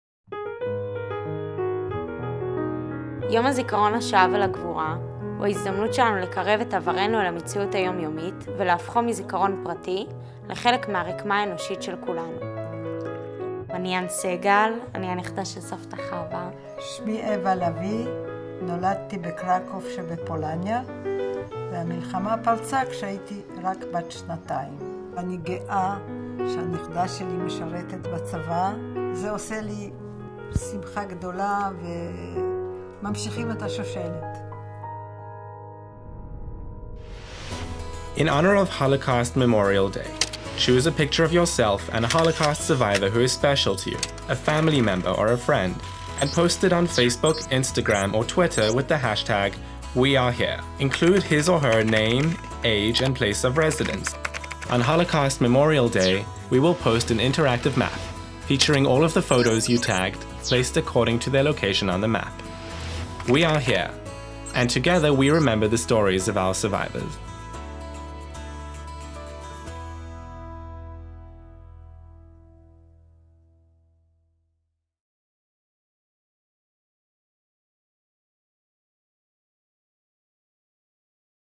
WeAreHere: Conmemoramos a las víctimas del Holocausto en las redes sociales
Una de las razones por la cual las FDI fueron creadas, es para que nunca jamás se vuelvan a repetir las atrocidades que el pueblo judío pasó en el Holocausto. Con motivo del Día de conmemoración a las víctimas de la barbarie nazi, el ejército israelí creó una campaña especial en las redes sociales con el fin de reavivar la historia de los sobrevivientes y la memoria de los caídos. Encuentra la manera de participar en este video instructivo.